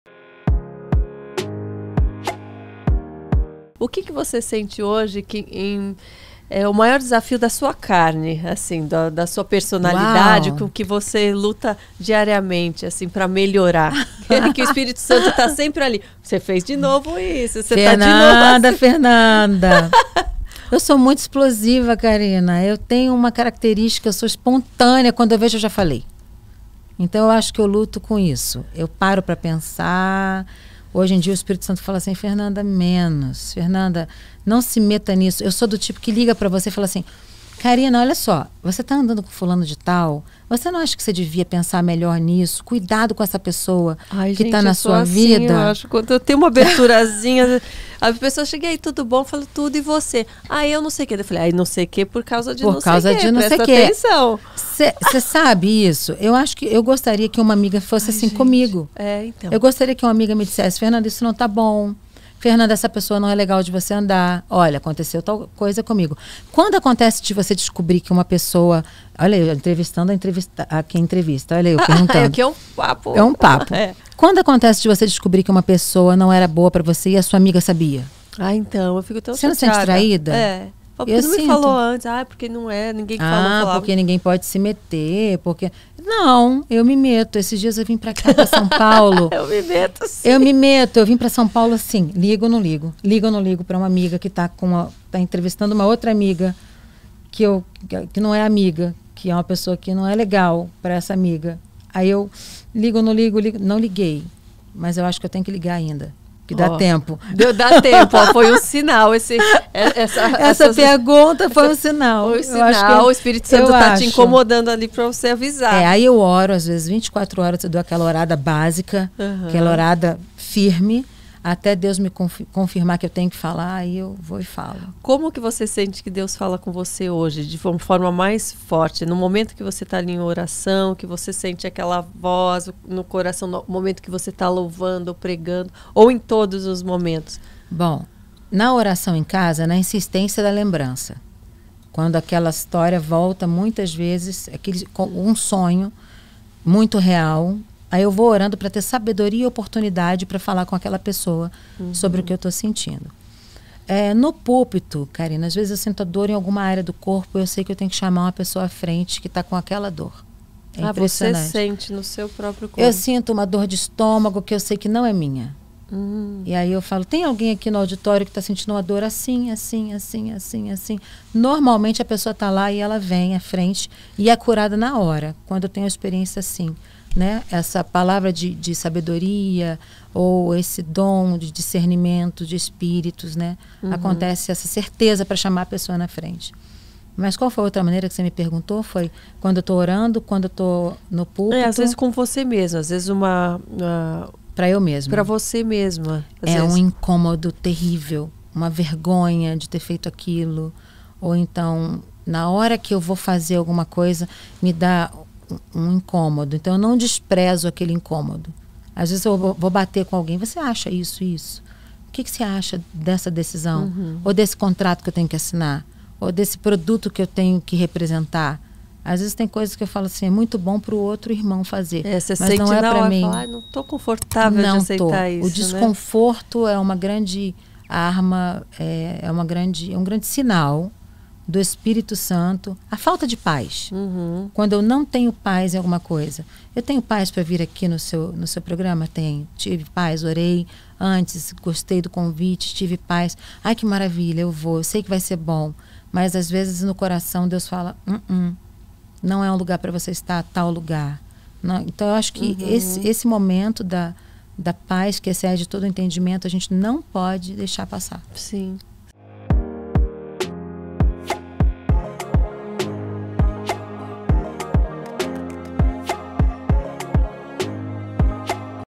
FERNANDA BRUM REVELA MAIORES DESAFIOS NA SUA PERSONALIDADE | POSITIVAMENTE PODCAST | PLANET GOSPEL
PODCAST DO CANAL : Positivamente Podcast